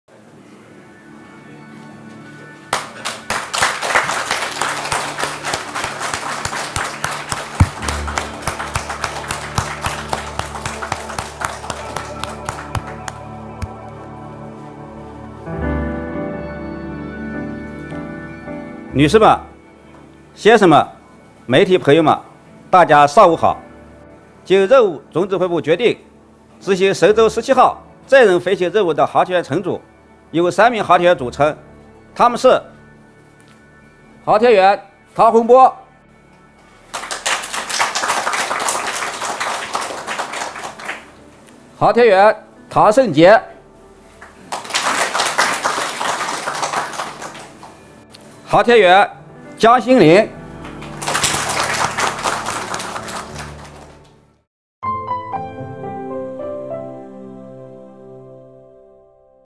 Meet China's youngest astronaut crew of Shenzhou-17 mission
Chinese astronauts Tang Hongbo, Tang Shengjie and Jiang Xinlin will carry out the Shenzhou-17 manned spaceflight mission to the China Space Station (CSS), and Tang Hongbo will be the commander, the China Manned Space Agency (CMSA) announced at a press conference on Wednesday.

The three Chinese astronauts for the upcoming Shenzhou-17 spaceflight mission, met the press at the Jiuquan Satellite Launch Center in northwest China.